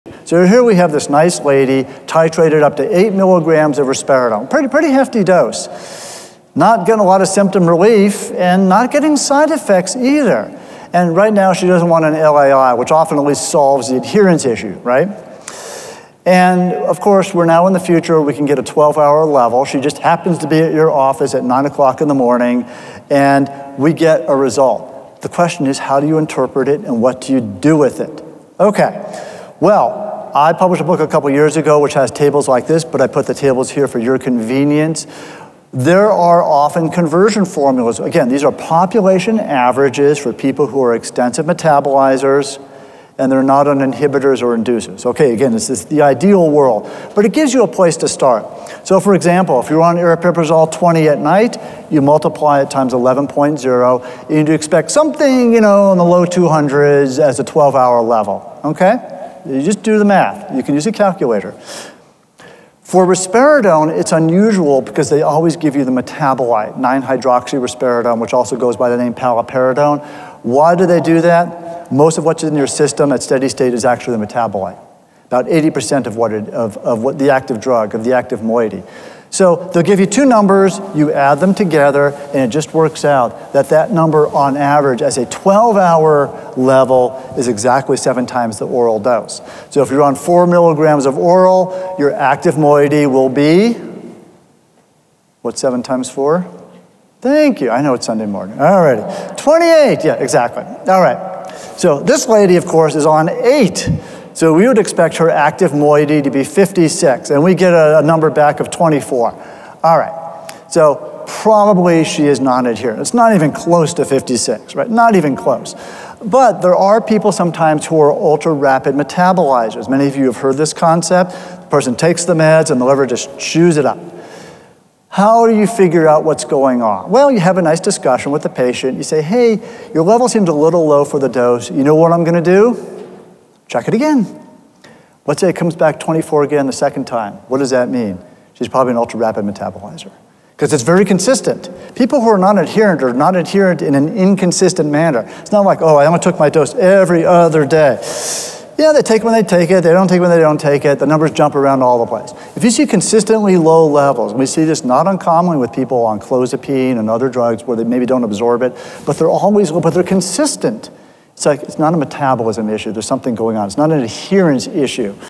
Plasma Levels and Antipsychotic Adherence
When it comes to individuals with schizophrenia who are on an antipsychotic regime, nonadherence is a prevalent and real barrier to successful treatment outcomes. Here, Dr. Meyer explains how to determine whether low plasma levels are a nonadherence vs metabolism issue, which is often difficult to spot.

Presented at the 2023 NEI Congress, session titled, "Level Up! Using Plasma Levels, Pharmacokinetics, and Pharmacodynamics to Guide Antipsychotic Prescribing". NEI Members can watch the full lecture